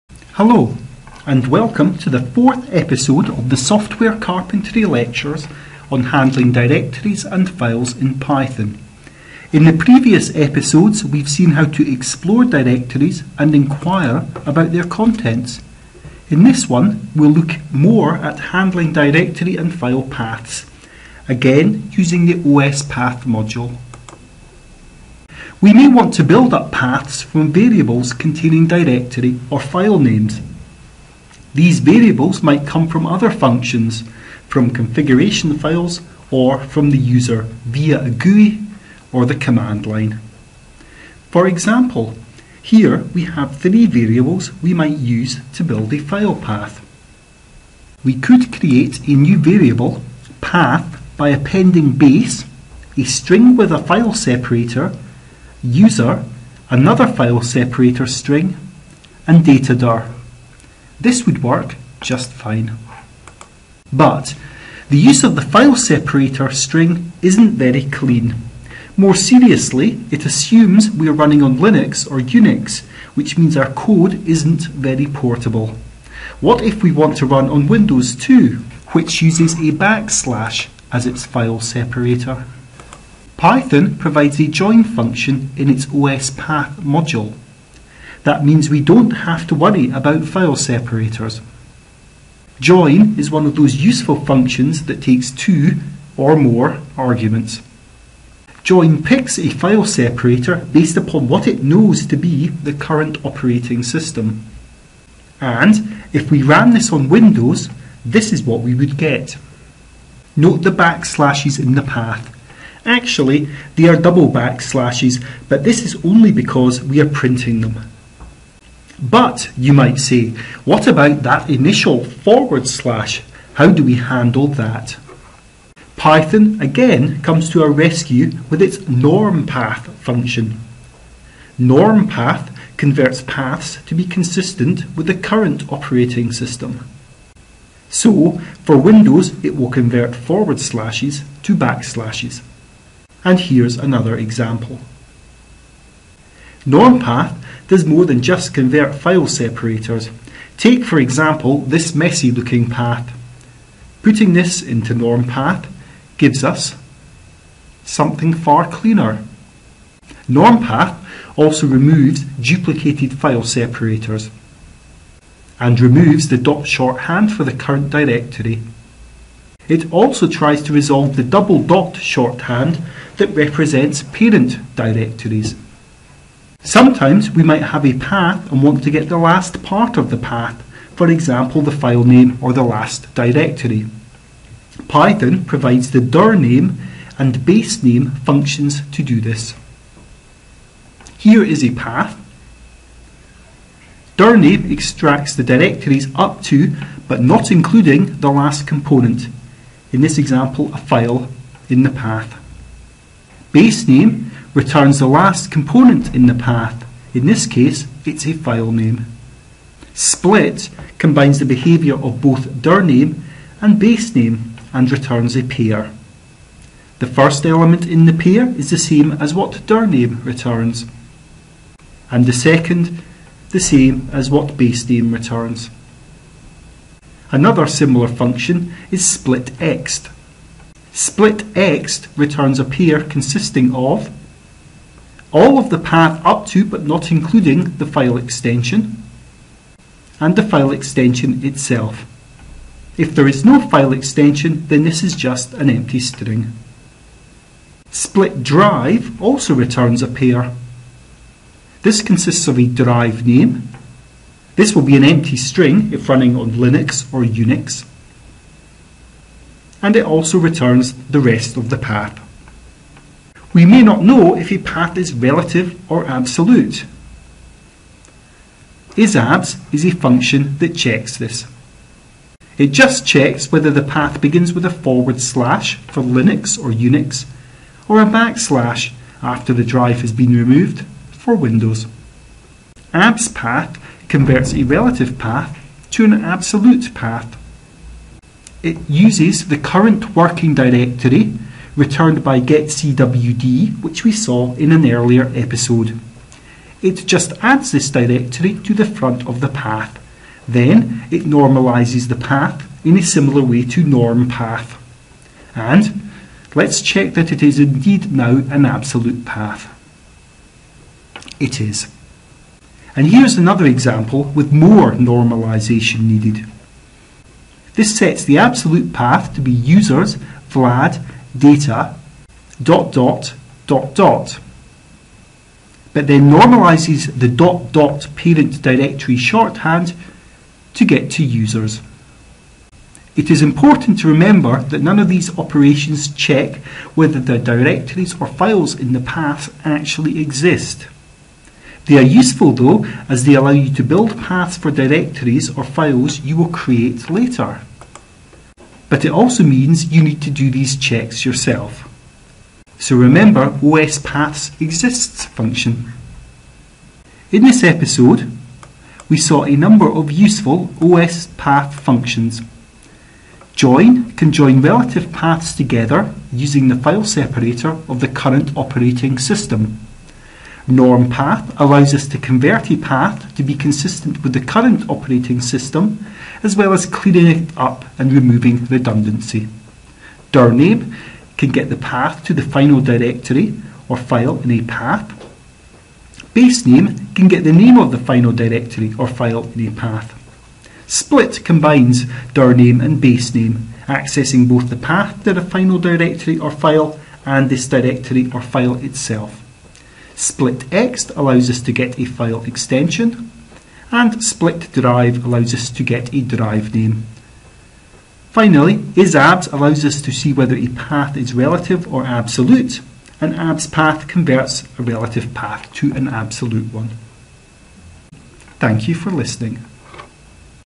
Systems Programming - Episode 4 - Directory and File Paths
Hello and welcome to the fourth episode of the Software Carpentry lectures on handling directories and files in Python. In the previous episodes, we've seen how to explore directories and enquire about their contents. In this one we'll look more at handling directory and file paths, again using the os.path module.